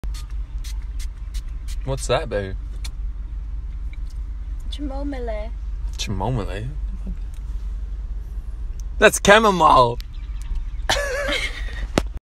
Chamomile Mispronunciation TikTok Video Original (Chamomalay)
The Original Chamomile mispronunciation Video from TikTok
'CHAMOMALAY'